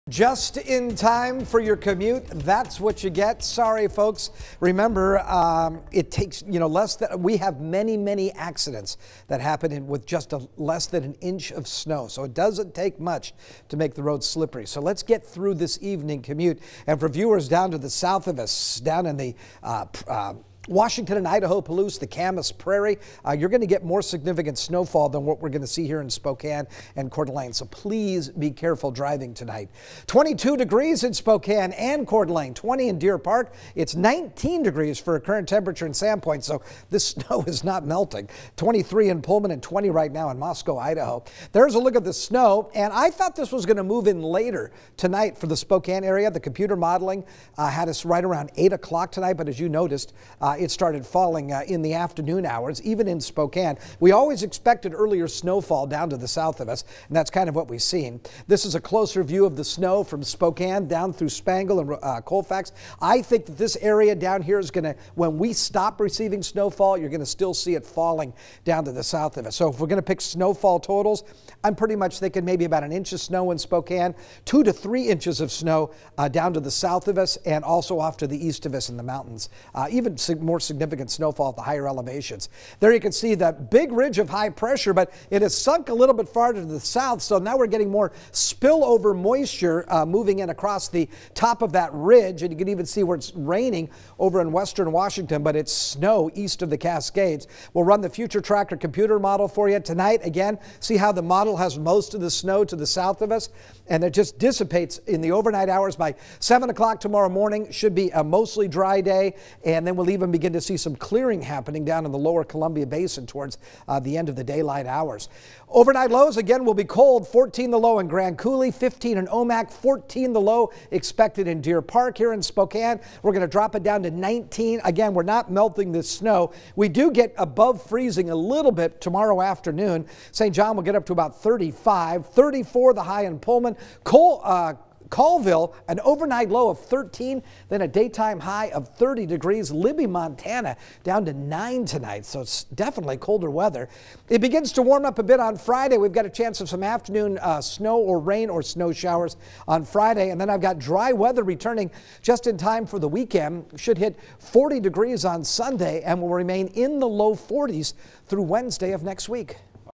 Winter Weather Advisory in place for the Palouse
Winter Weather Advisory remains in effect from 4 p.m. to 4 a.m for the Idaho Palouse areas. Spokane could pick up an inch of snow by Thursday morning.